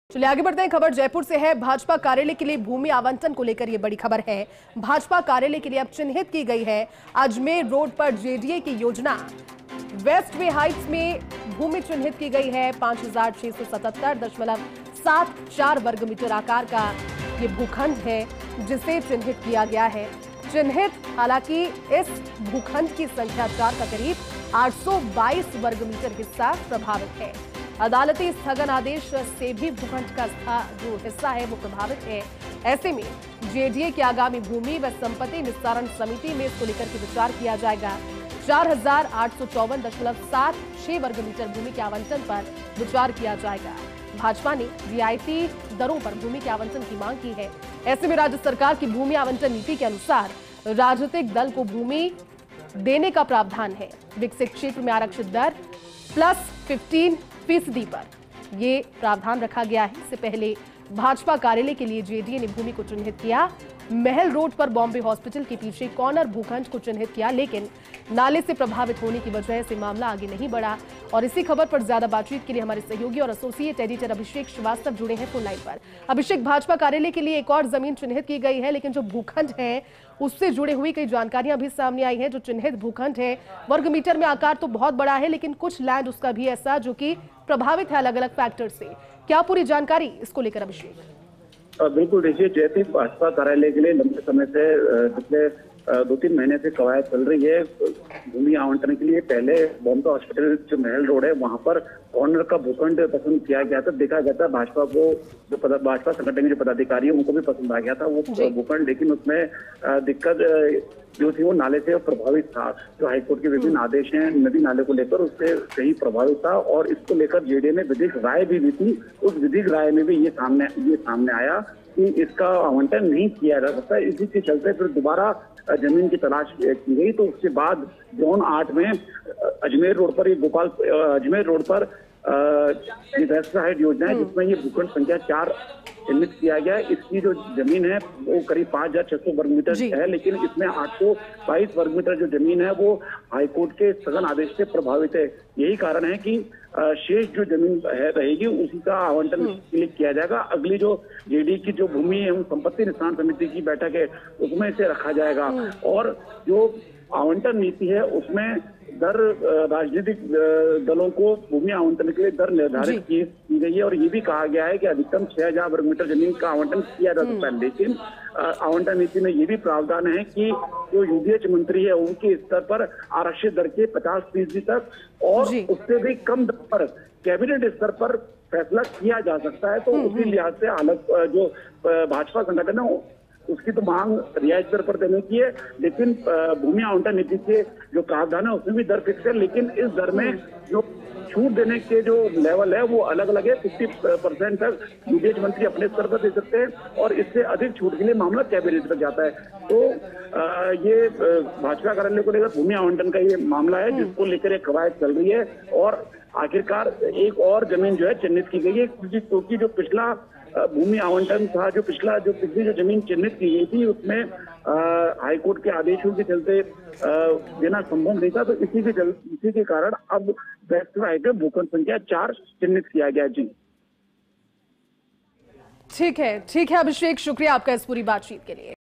BJP कार्यालय के लिए भूमि आवंटन को लेकर बड़ी खबर | Jaipur | Rajasthan News
About this Video:
जयपुर: भाजपा कार्यालय के लिए भूमि आवंटन को लेकर बड़ी खबर, भाजपा कार्यालय के लिए अब चिन्हित की गई है भूमि, अजमेर रोड पर जेडीए की योजना वेस्ट वे हाइट्स में चिन्हित की भूमि, 5677.74 वर्गमीटर आकार का भूखंड किया गया है चिन्हित, हालांकि इस भूखंड संख्या 4 का करीब 822 वर्गमीटर हिस्सा है प्रभावित, अदालती स्थगन आदेश से भूखंड का यह हिस्सा है प्रभावित, जेडीए की आगामी भूमि व संपत्ति निस्तारण समिति में किया जाएगा विचार, 4854.76 वर्गमीटर भूमि के आवंटन पर किया जाएगा विचार, भाजपा ने रियायती पर भूमि के आवंटन की मांग की है, राज्य सरकार की भूमि आवंटन नीति के मुताबिक, राजनीतिक दल को भूमि भूमि देने का है प्रावधान, विकसित क्षेत्र में आरक्षित दर+15% दर पर देने का है प्रावधान, इससे पहले भाजपा कार्यालय के लिए जेडीए ने चिन्हित की थी भूमि, महल रोड पर बॉम्बे हॉस्पटिल के पीछे कार्नर भूखंड किया था चिन्हित, लेकिन नाले से प्रभावित होने के कारण मामला नहीं बढ़ा आगे, | 24 October 2024